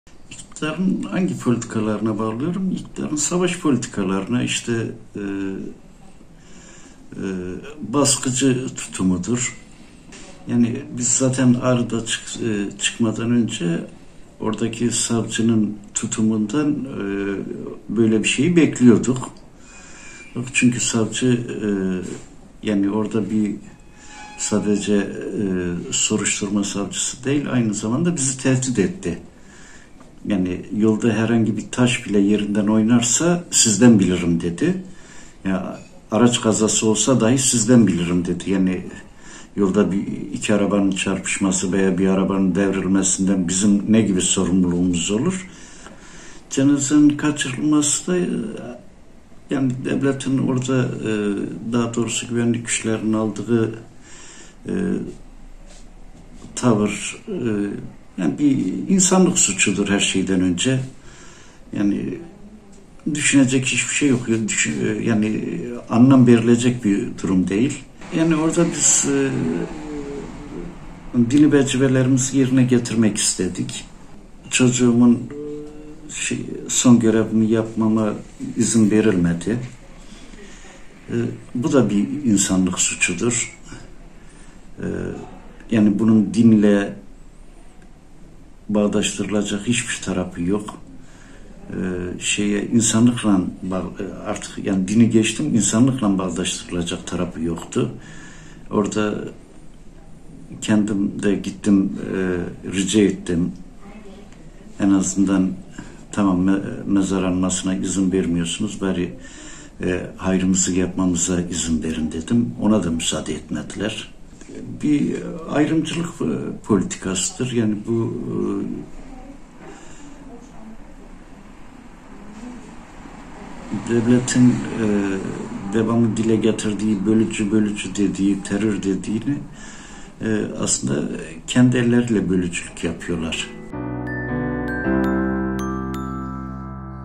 ‘CENAZELERE SALDIRI KÜRTLERE YÖNELİK AYRIMCI POLİTİKADAN KAYNAKLANIYOR’
Ulaş Alankuş’un cenazesine yönelik saldırıları insanlık suçu olarak tanımlayan babası Semih Alankuş, saldırının iktidarın Kürtlere yönelik ayrımcı politikasından kaynaklandığını belirtti.